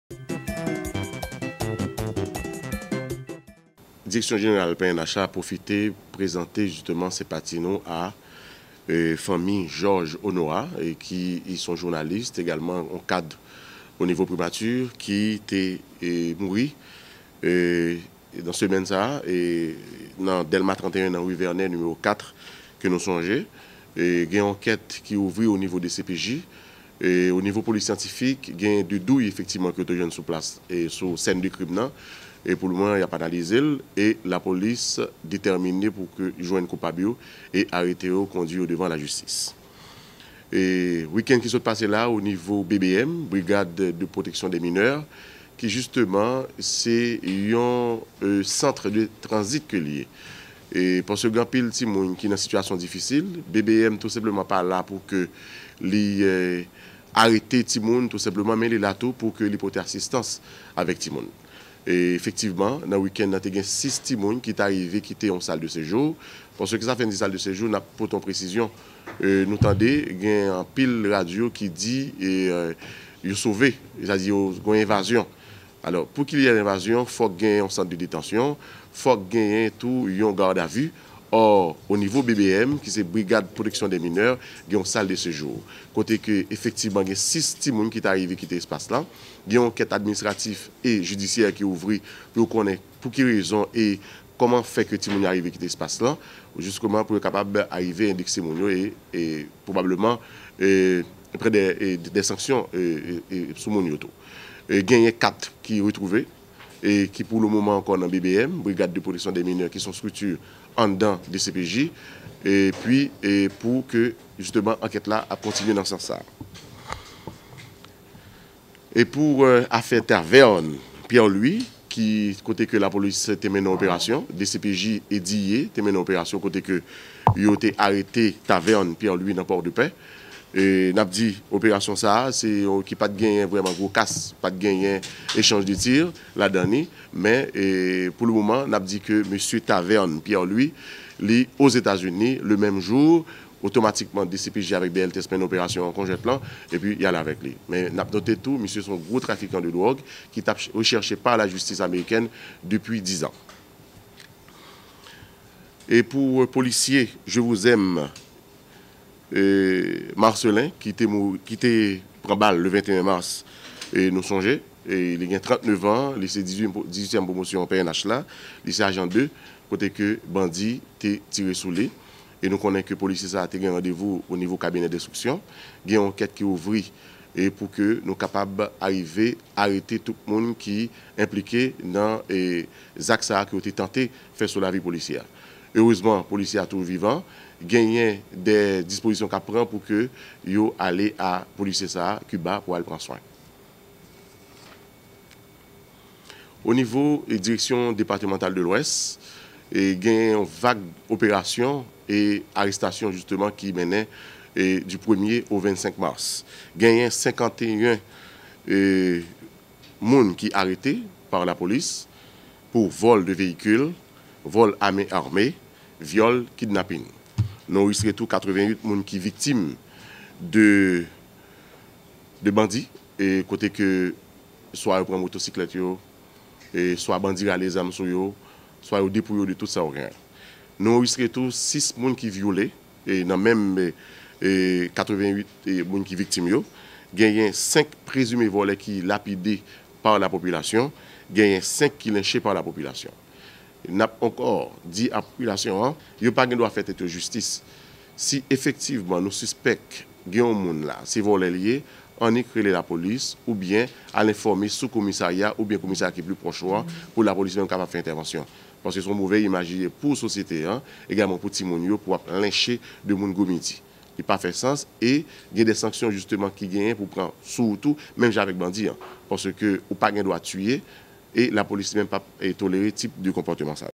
Bilan operasyon Polis nasyonal la reyalize nan zòn metwopolitèn soti premye mas pou rive 25 mas.
Bilan operasyon Polis nasyonal la reyalize nan zòn metwopolitèn soti premye mas pou rive 25 mas 2013 la.